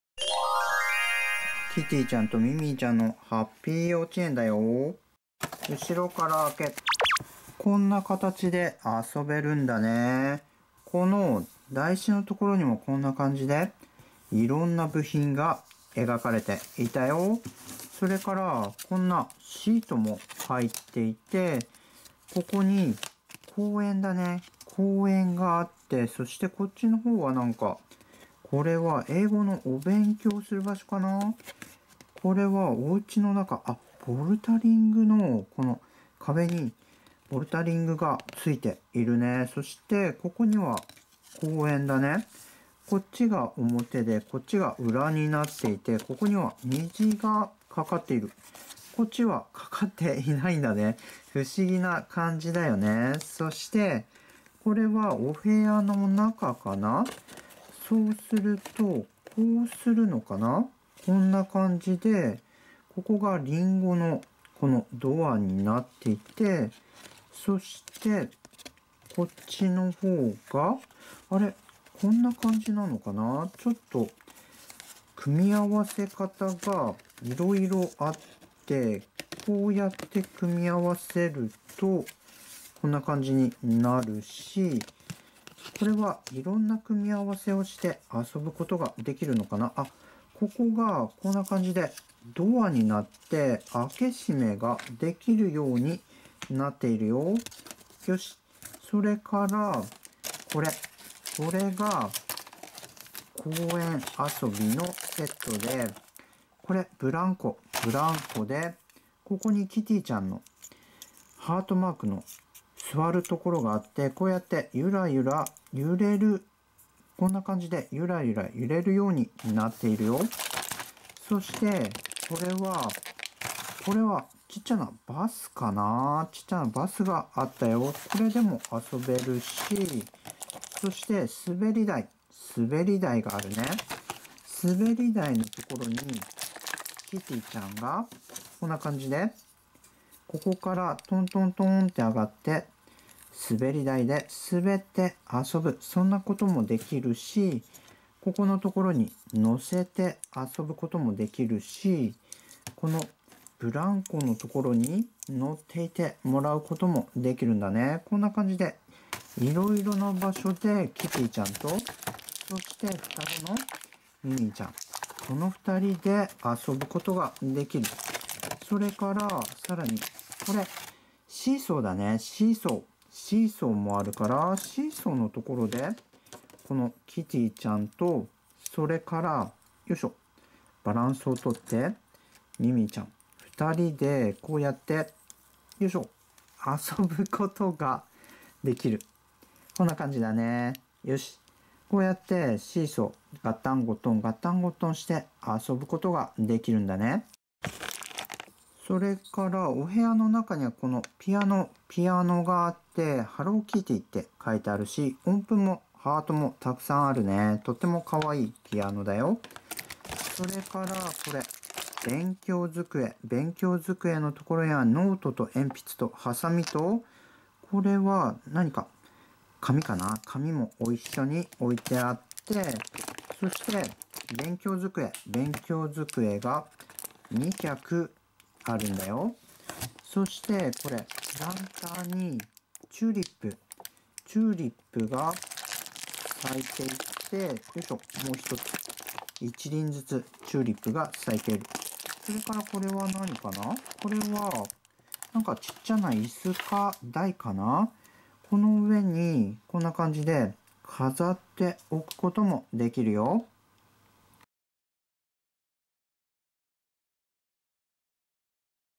hello kitty miniature toys ハローキティのちいさなおもちゃがいっぱい NEWハッピーようちえんセット
It's a toy's clip dayo.おもちゃの動画"公園で遊ぶブランコ、すべり台、シーソー、バスや、ピアノ、机と椅子があるん"だよ。

■つぎはこれをみようかな？

●ままごとトントン おりょうりおもちゃセット